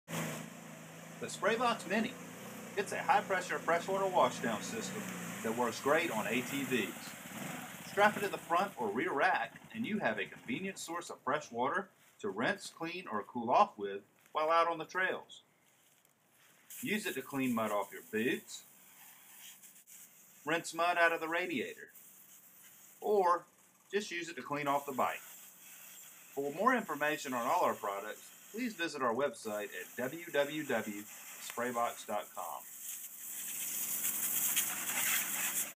Spray Box Mini - ATV / UTV accessory
ATVing with the Spray Box Mini. -  Provides a convenient source of high-pressure freshwater while out on the trails. Fits on ATV's UTV's, Jeeps, jon boats, etc. A portable source of freshwater while away from home.